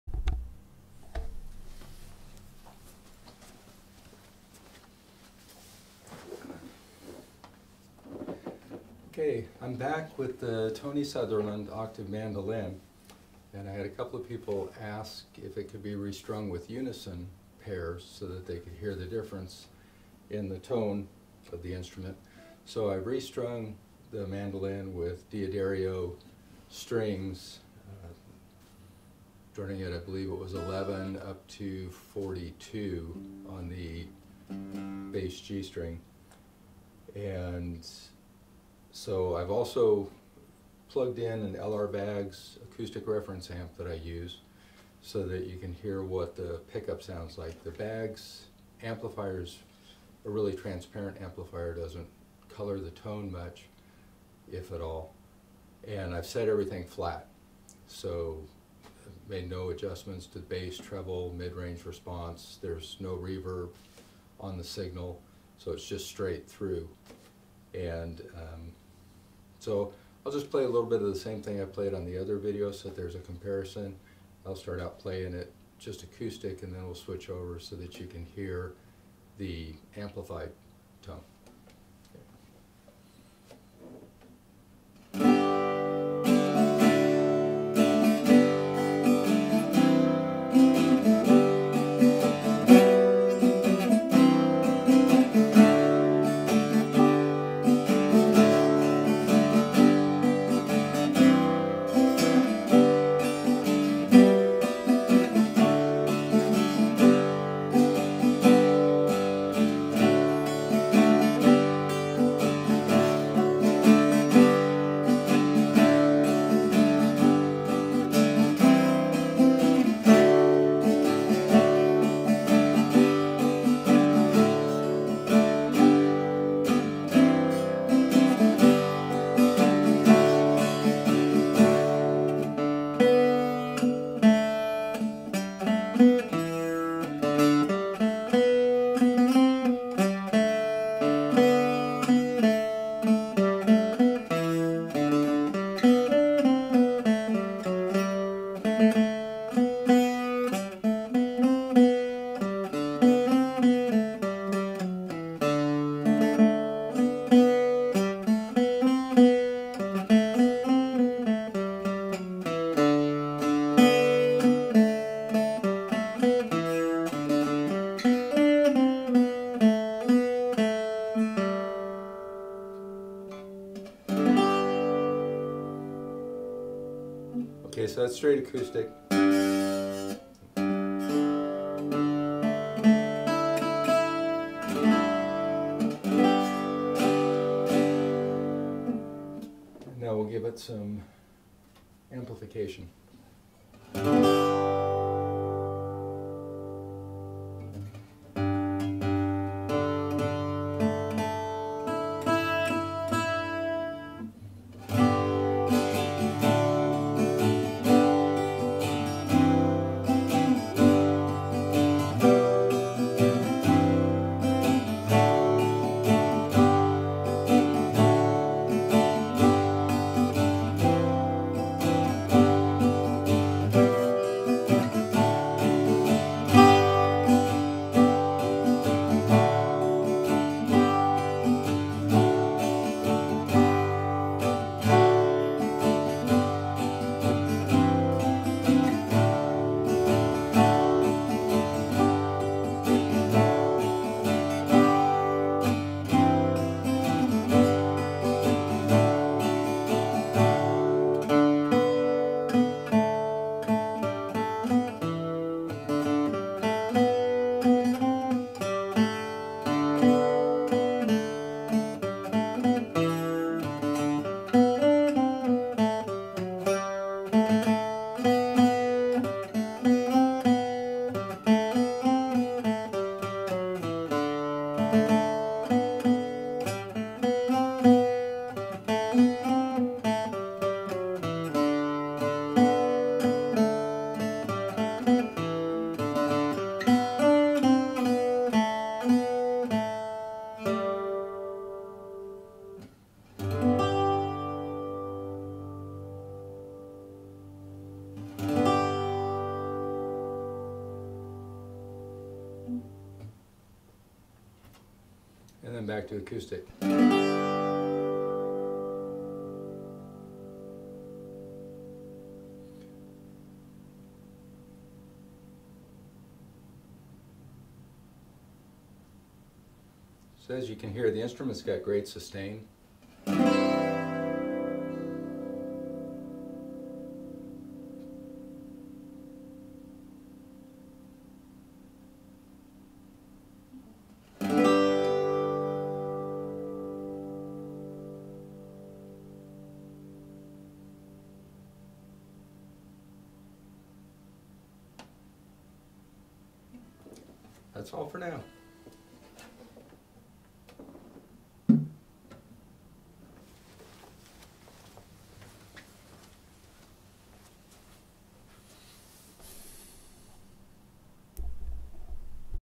Tony Sutherland Octave Mandolin demo
Tony Sutherland GBOM strung with unison pairs,  acoustic and then with the K&K pickup run through a LR Baggs Acoustic Reference amp